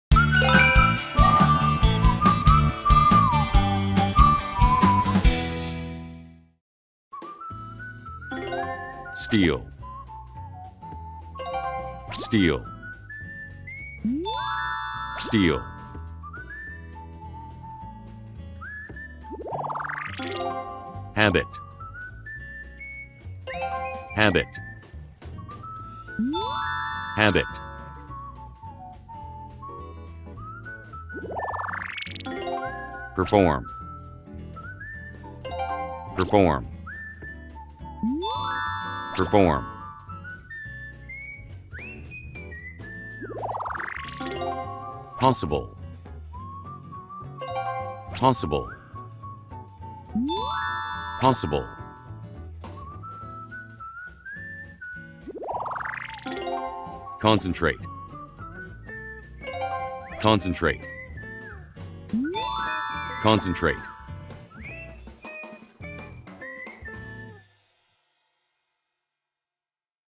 [1촉즉발 VoCard] 24주차. steal | habit | perform | possible | concentrate 각 단어의 의미와 품사 공부하러 출-발.
영어 공부의 시작은 영어 단어를 아는 것부터! 1분 안에 스피-디하게 5개 단어 암기!

더 많은 강의를 보고 싶다면?